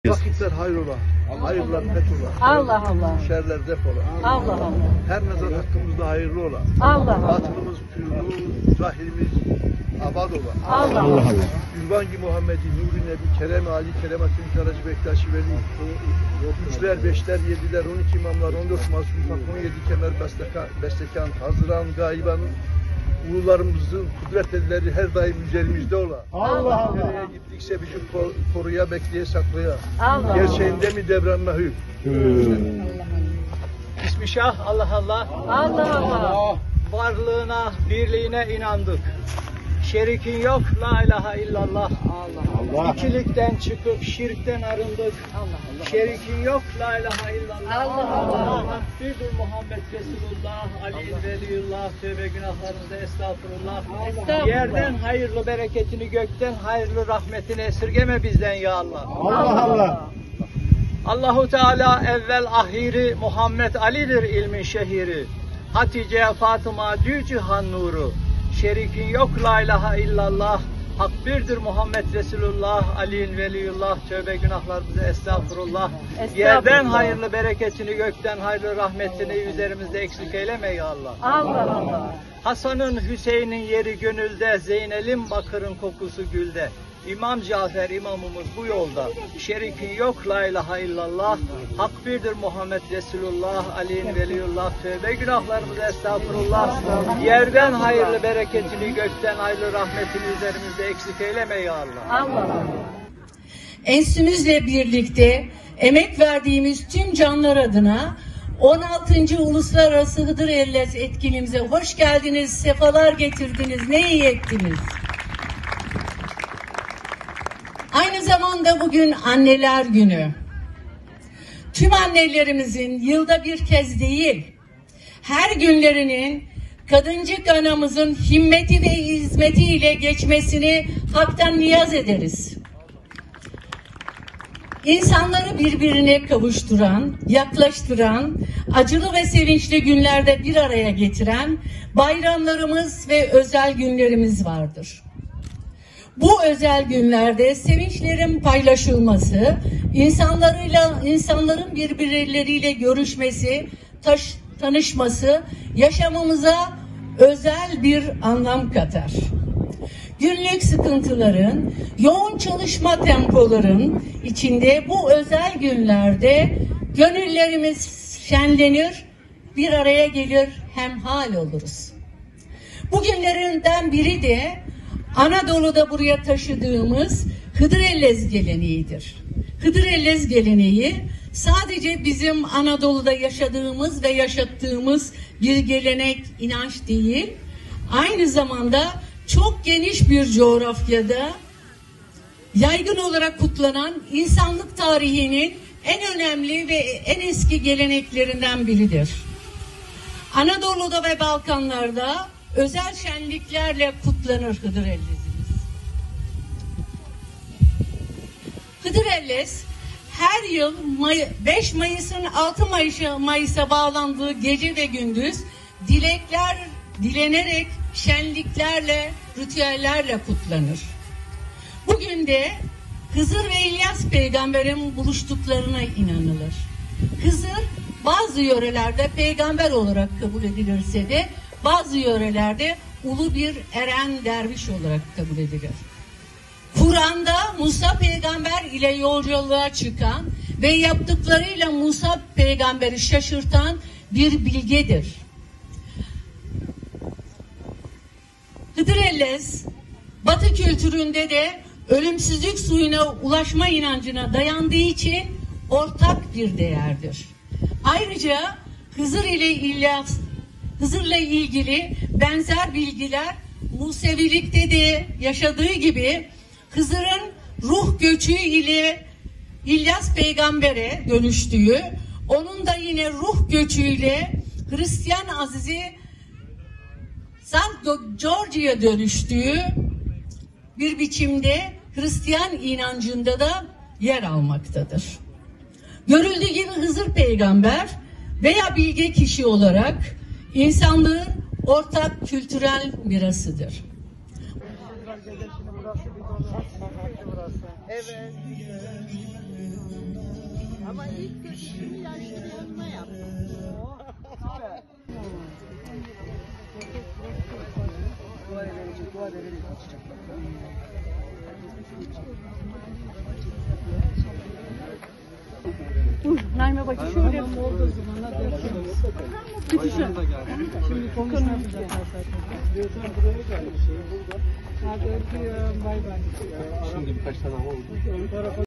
Almanya’da 16. Hıdırellez etkinliği düzenlendi
PİRHA - Alevi Bektaşi Kültür Enstitüsü, 16. Uluslararası Hıdırellez Etkinliği düzenledi. Yapılan kutlamada, Hıdırellez geleneğine dair aktarımlar yapılırken, nefesler de seslendirildi.